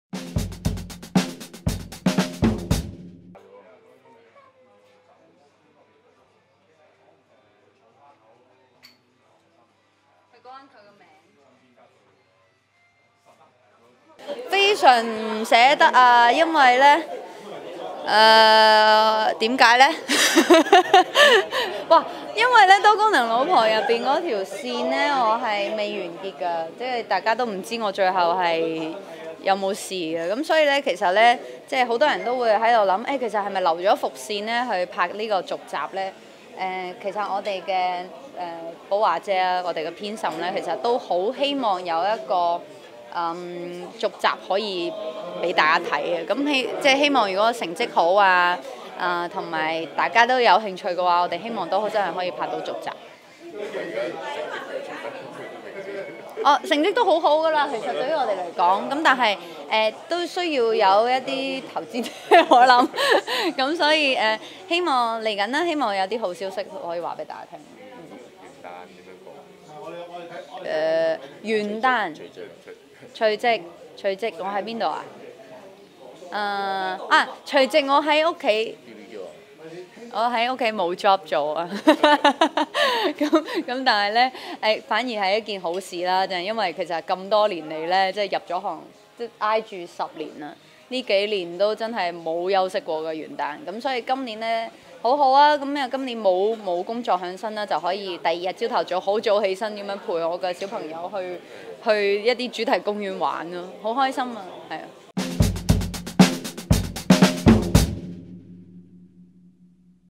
【廣東話】爆《多功能老婆》埋下伏線 吳若希：好希望開到續集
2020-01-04【廣東話】爆《多功能老婆》埋下伏線 吳若希：好希望開到續集